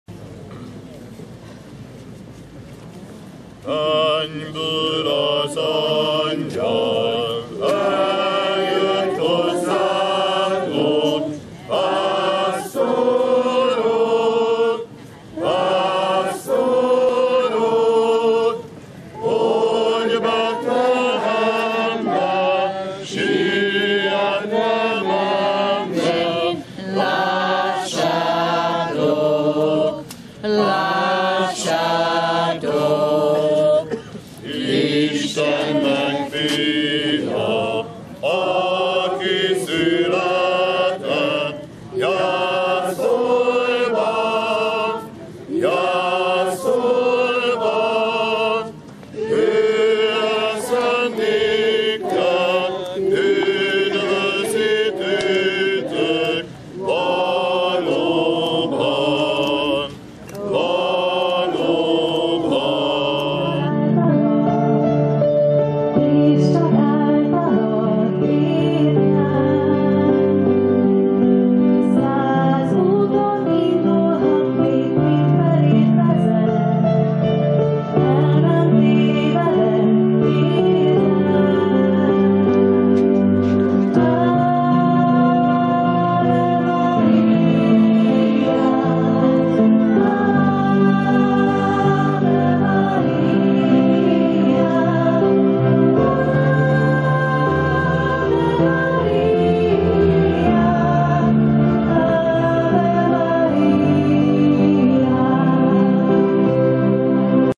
Betlehemes játék Máriapócson - szon.hu
Hagyományosan a templom előtti téren szórakoztatták az érdeklődőket a Pócsi karácsony - című betlehemes játékkal, amelyben napi aktualitásokkal tréfás kiszólásokkal és dalbetétekkel kiegészített előadás aratott sikert.